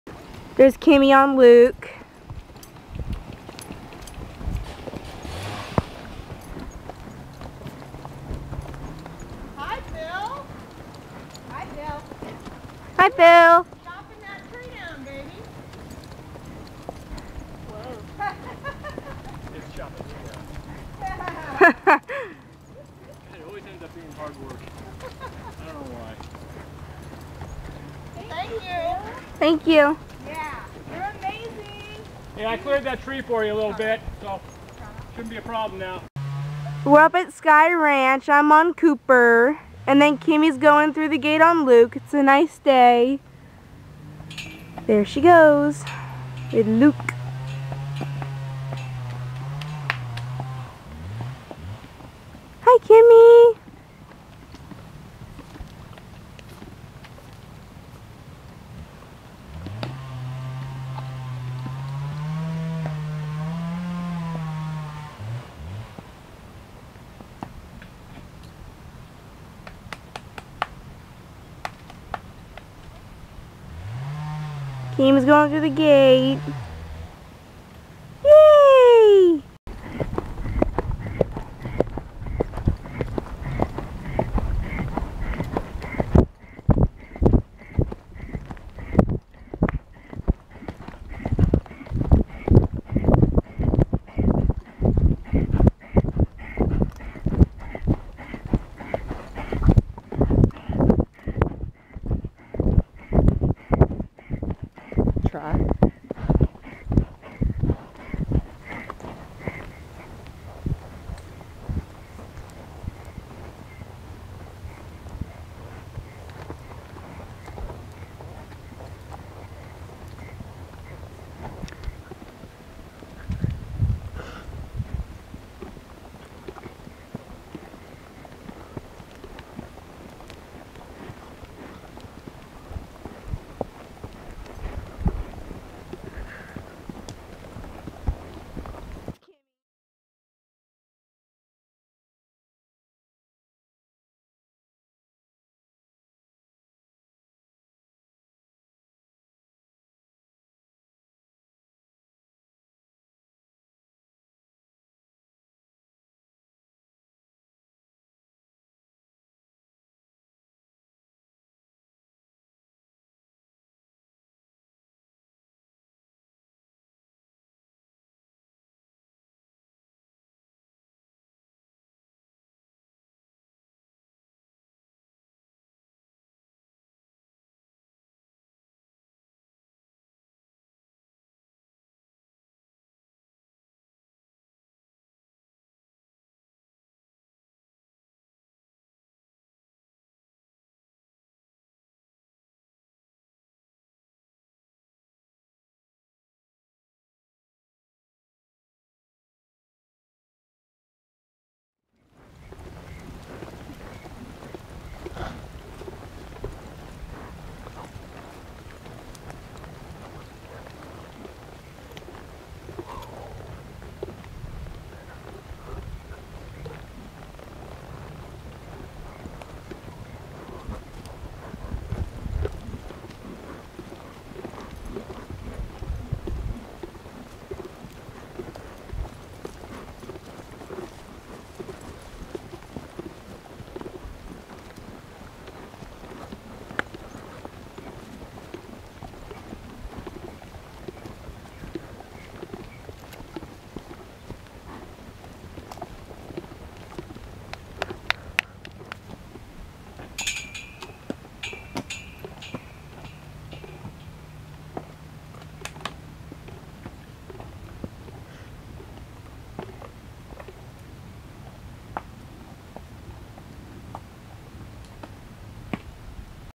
Neigh Savers - Luke on the Trail
Luke showing off his skills riding in front, beside and WAY behind other horses.  Herding cattle, cantering and encountering a man with a chainsaw.